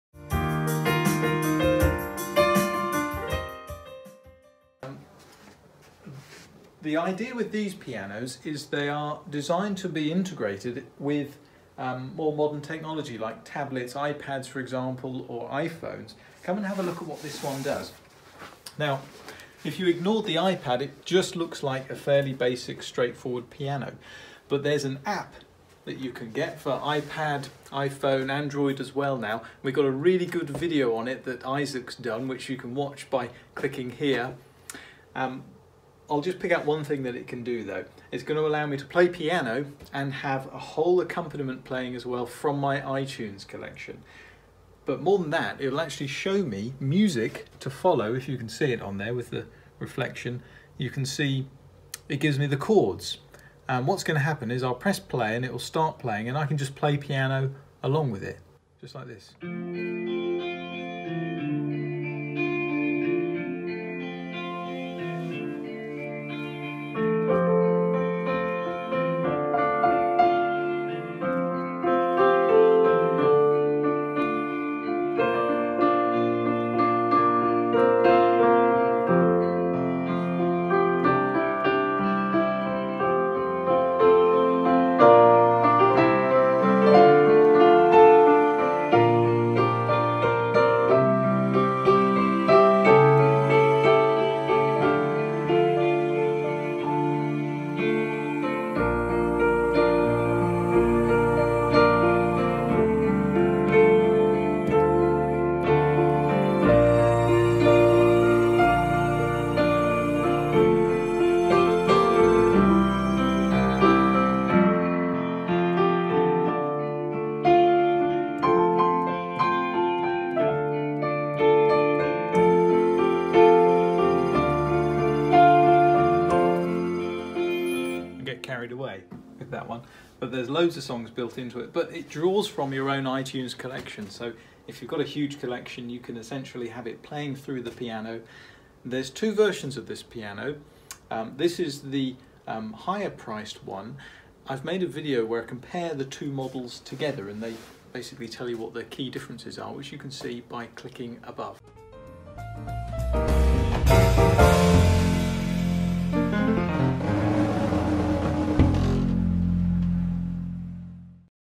Yamaha CSP170 Clavinova demonstration | Showroom Live highlights
Chris gives a short demonstration of the Yamaha CSP-170 Clavinova smart piano. He uses a connection to an iPad to play along to a backing track while following music on the screen.

This video is a highlight from our regular 'Showroom Live' series.

for the UK's best deals on Yamaha digital pianos and keyboards.

Delivery is FAST and FREE across the UK.